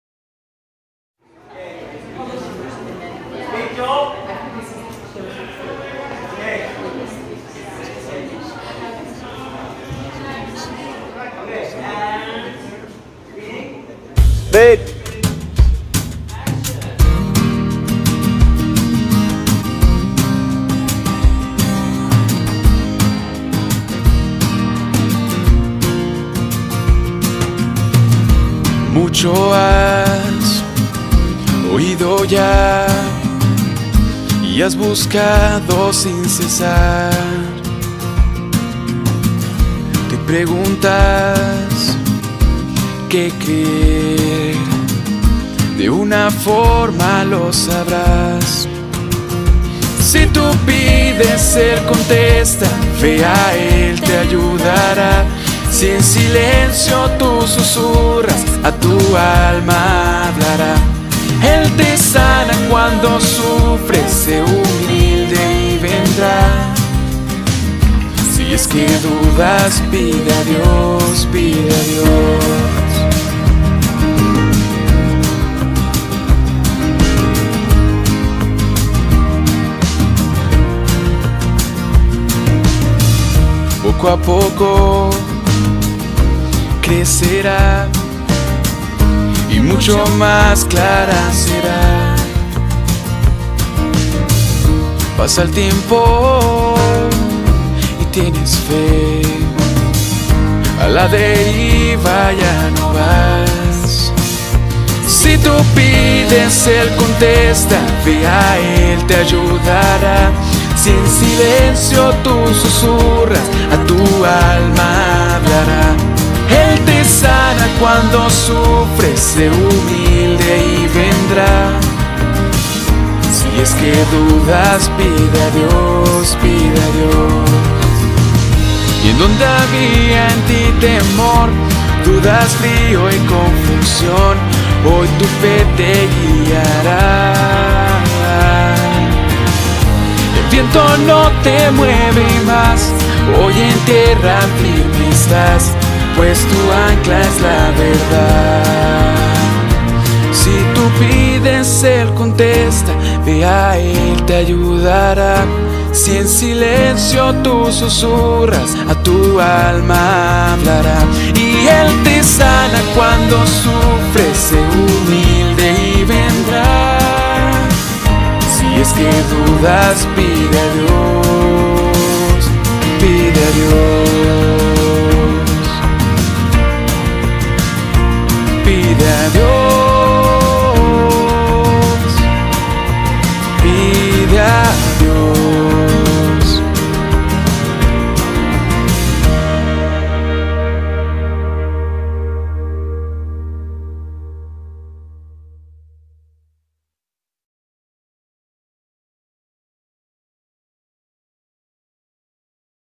LEMA DE LA MUTUAL 2017 - PIDE A DIOS - MENSAJES SUD
“Y si alguno de vosotros tiene falta de sabiduría, pídala a Dios, quien da a todos abundantemente y sin reproche, y le será dada.
Pero pida con fe, no dudando nada, porque el que duda es semejante a la ola del mar, que es movida por el viento y echada de una parte a otra” (Santiago 1:5–6).